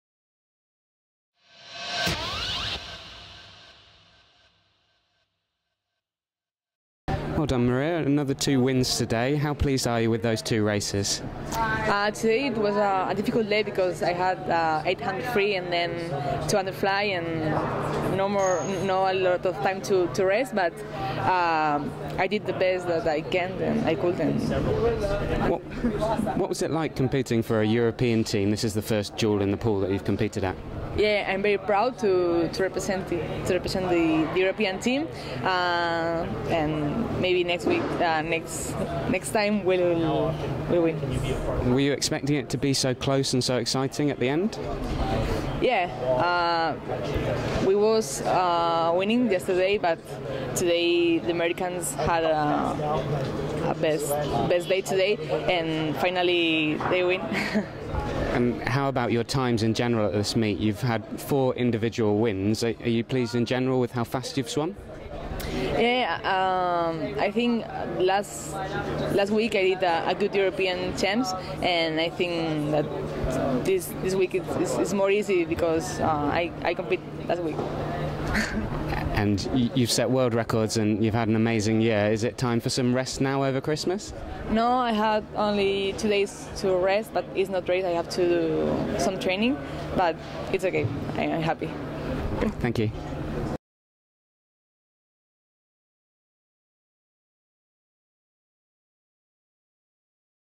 Mireia Belmonte claims two individual wins on day two of the 2013 Speedo Duel In The Pool
Mireia Belmonte wins her third and fourth individual races of the 2013 Speedo Duel In The Pool as the European Allstars lose a tie-breaker for the title in Glasgow.